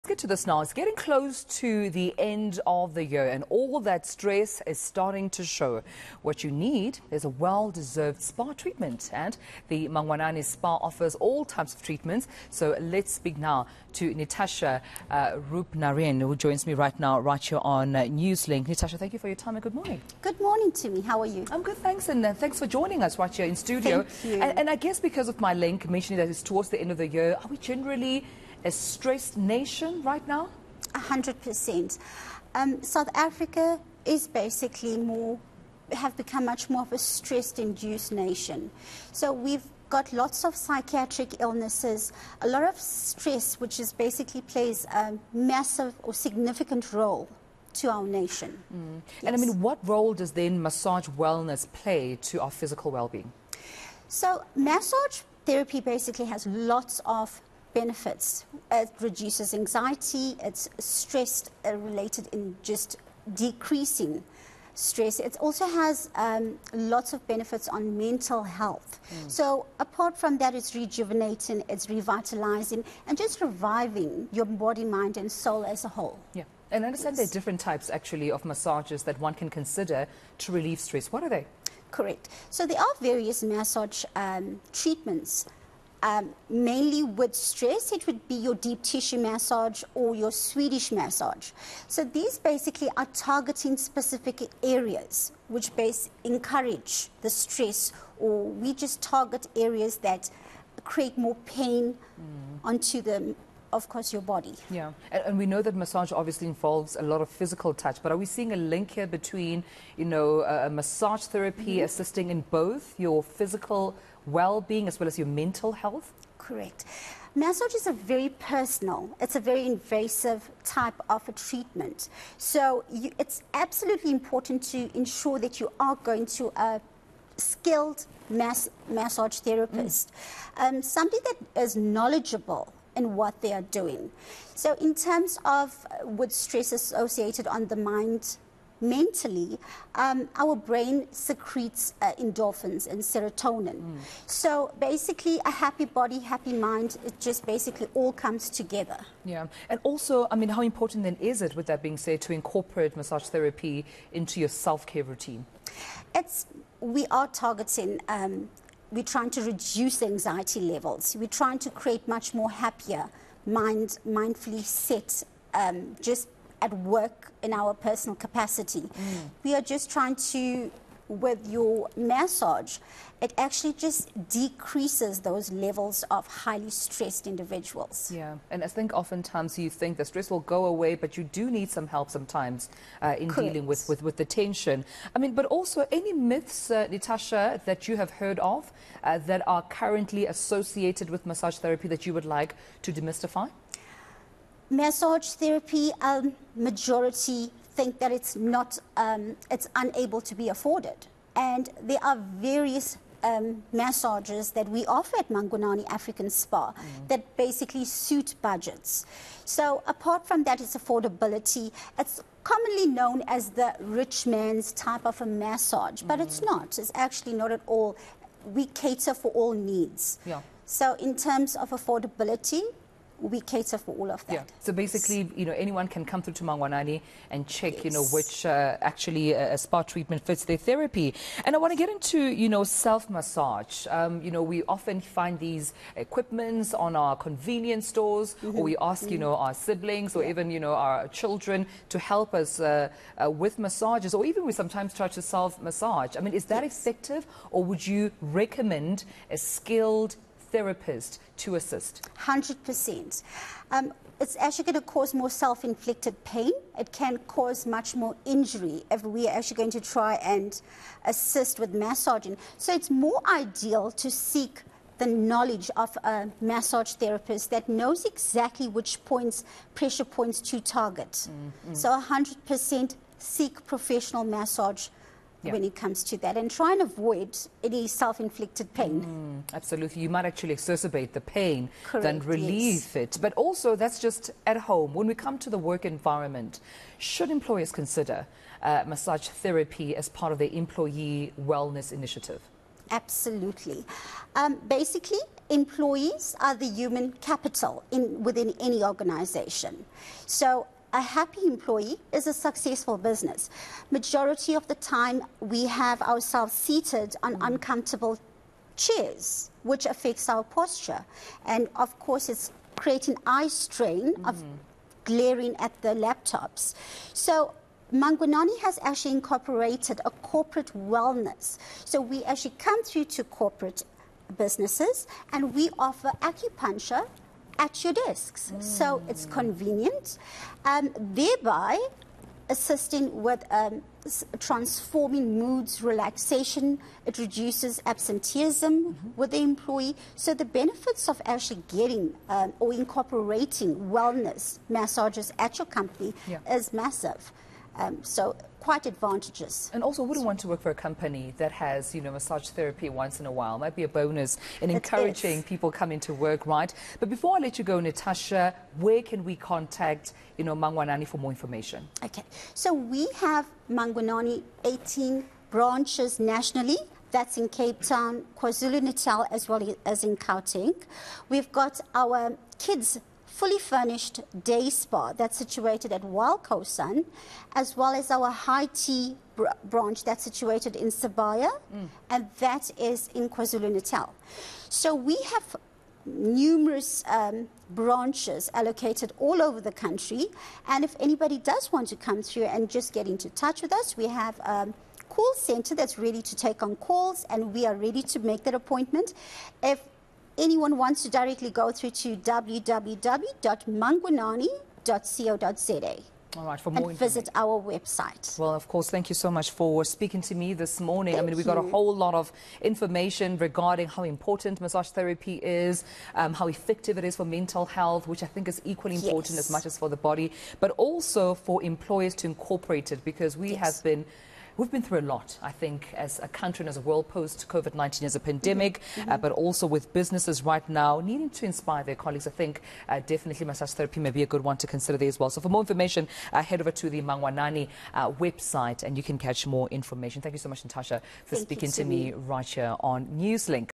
Massage Therapy | Keeping end of year stress at Bay
It's getting close to the end of the year, and all that stress is starting to show. What you need is a well-deserved spa treatment. We spoke to Nitasha Roopnarain from the Mangwanani Spa.